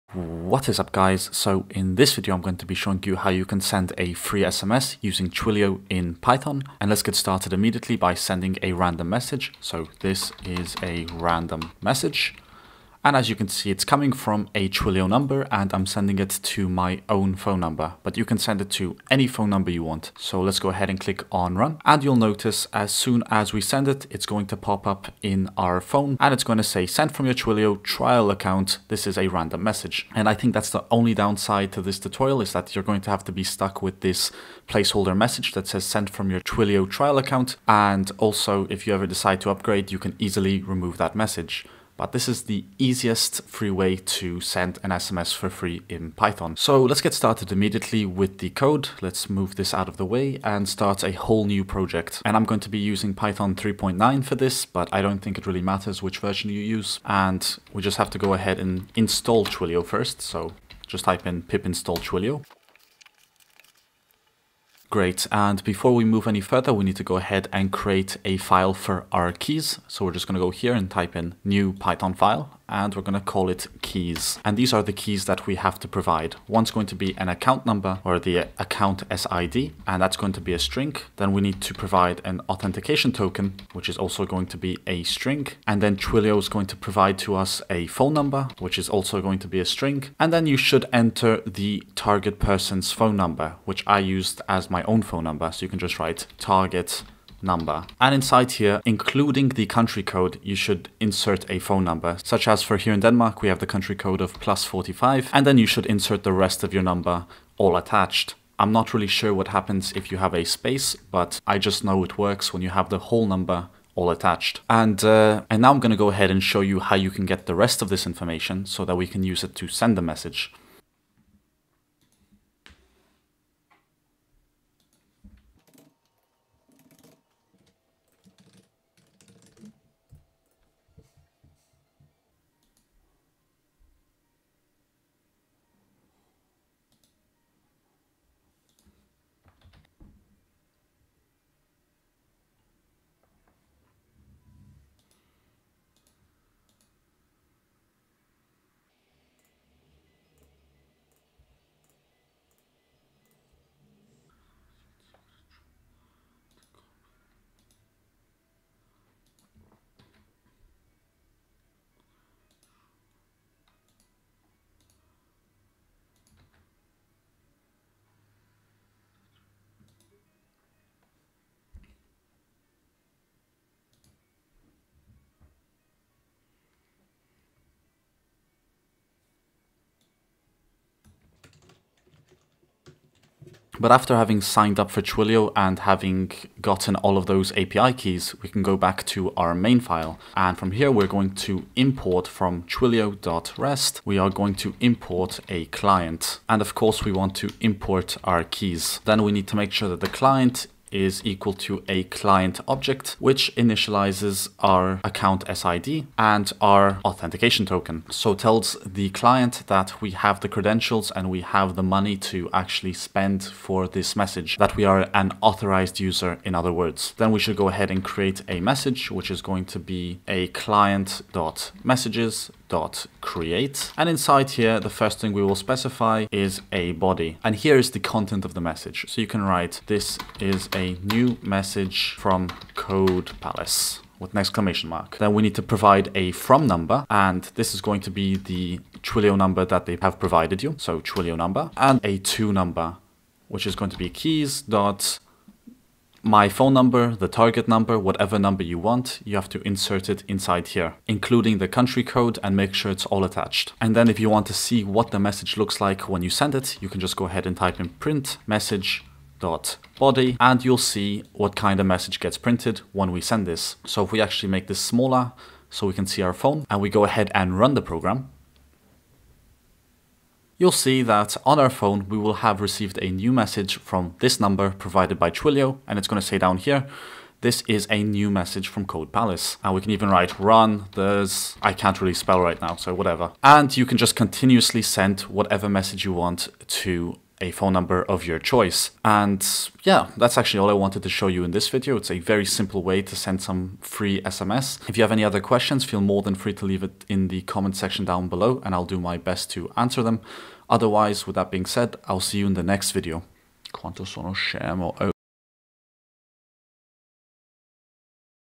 Send SMS for Free with Python Tutorial (Twilio)
In this video I'll show you how you can use Python (in combination with Twilio) to send free SMS messages to any phone number of your choice.